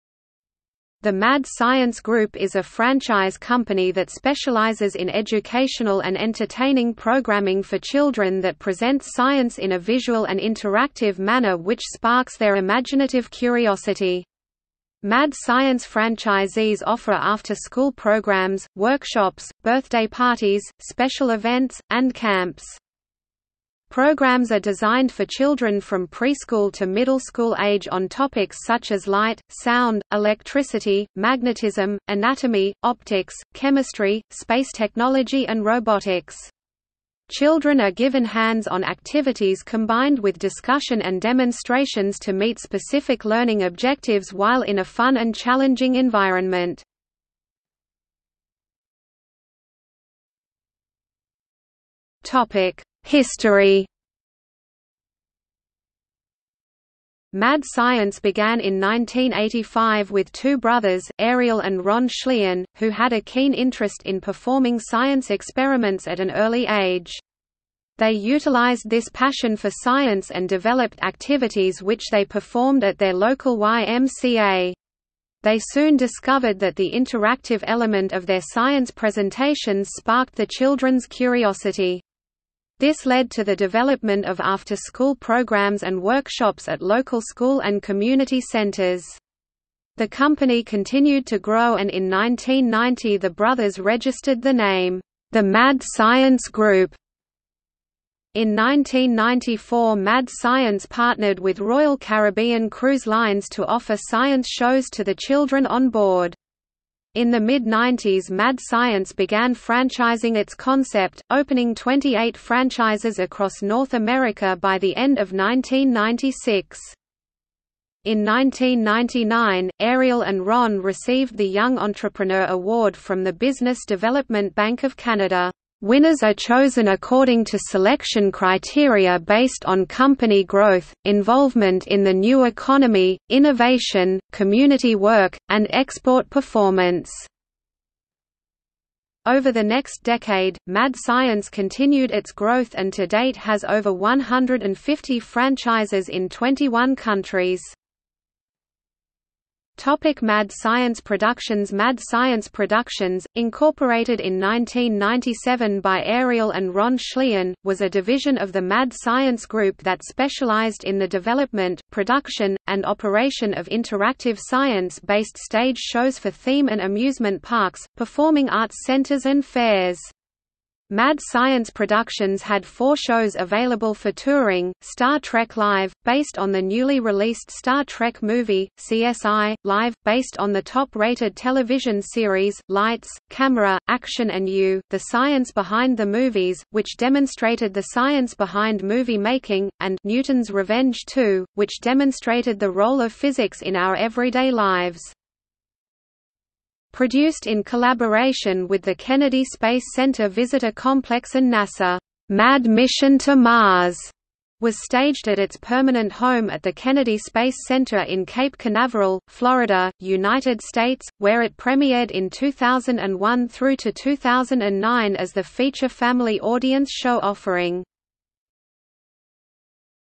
Mad Science | Wikipedia audio article
00:00:48 1 History
00:02:29 2 Mad Science Productions

- Socrates

SUMMARY
The Mad Science Group is a franchise company that specializes in educational and entertaining programming for children that presents science in a visual and interactive manner which sparks their imaginative curiosity.  Mad Science franchisees offer after school programs, workshops, birthday parties, special events, and camps.
Programs are designed for children from pre-school to middle school age on topics such as light, sound, electricity, magnetism, anatomy, optics, chemistry, space technology and robotics.  Children are given hands-on activities combined with discussion and demonstrations to meet specific learning objectives while in a fun and challenging environment.